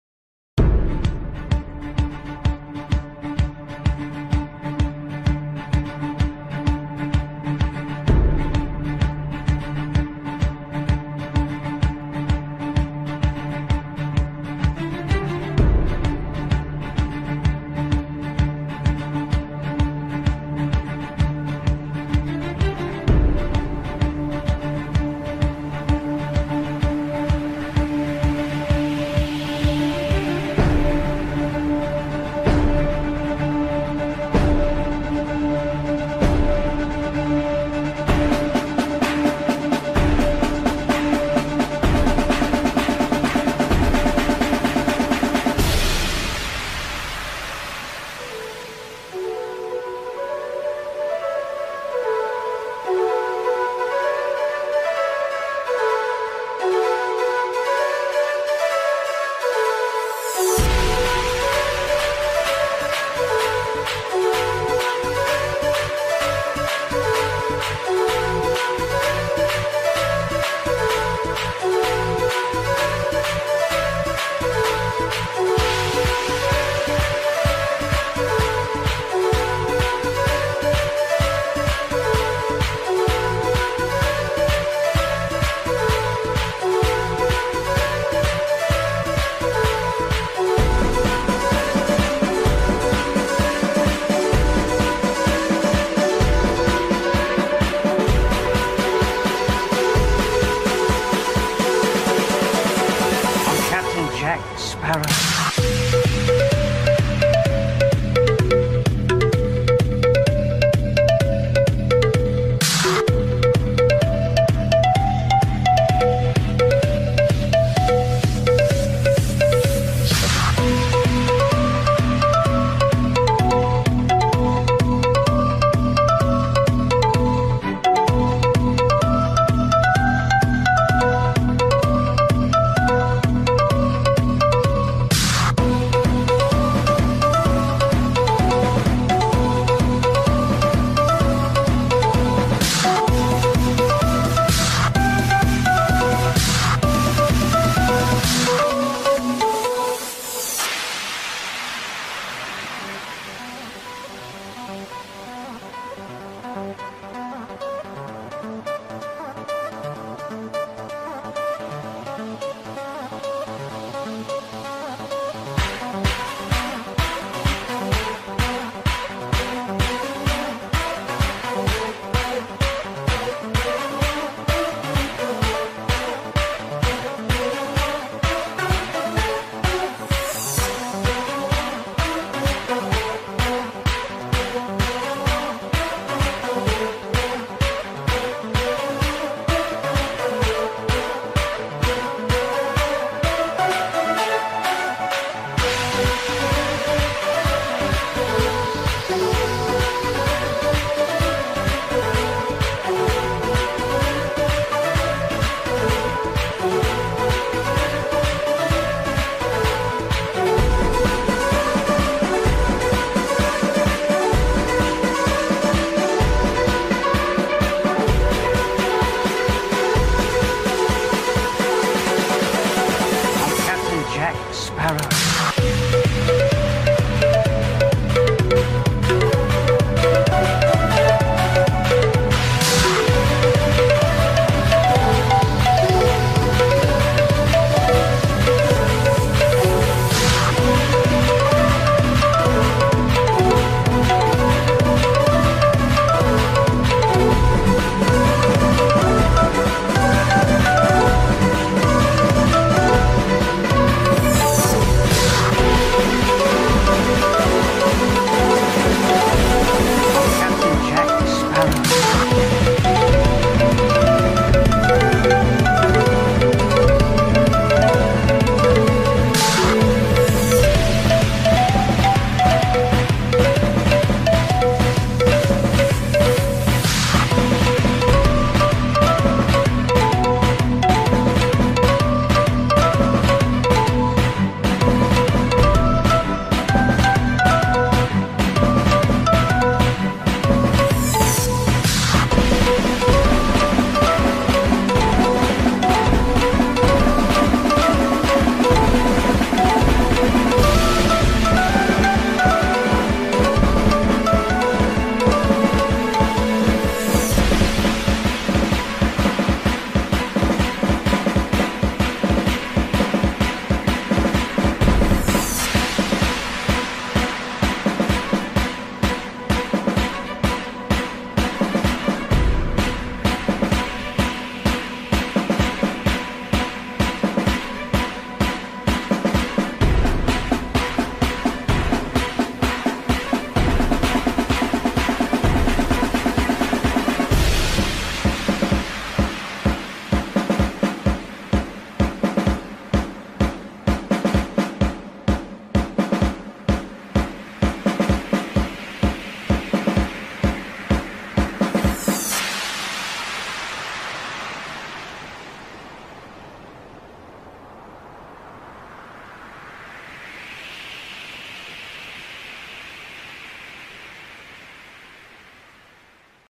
PVP corsair test! slippery when wet!
fast cooldown high mobility devastating attacks! corsair gushes through the arena like the roaring currents!